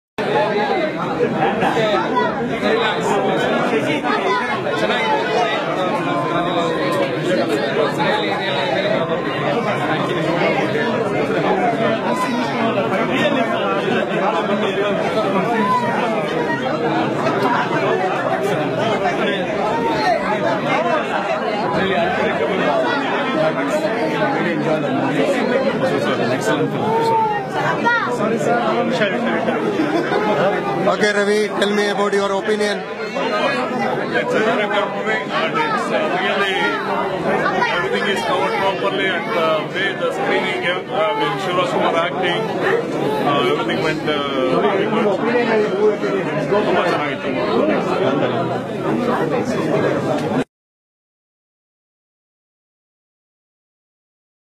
Shivalinga Movie reviews from London
Shivalinga Movie gets good responses from the London Kannadigaru
Dr.ShivaRajKumar jets into London for the UK Premier of Shivalinga (Kannada movie) with star cast of Shivalinga –Vedhika and director P. Vasu at Boleyn cinemas,Barking road, E6 1PW, London March 19th 2016. UK Premier of Shivalinga (Kannada movie) in London is a first-of-its-kind effort to promote South Indian Cinema globally.
Shivalinga is a 2016 Indian Kannada-language film written and directed by P. Vasu. It is a complete family entertainer and especially not to be missed by the fans of SRK.The film stars Shivarajkumar and Vedhika. Produced by K. A. Suresh, the film's music is composed by V. Harikrishna. The film was released on 12 February 2016.
Director: P. Vasu, Music composed by: V. Harikrishna ,Written by: P. Vasu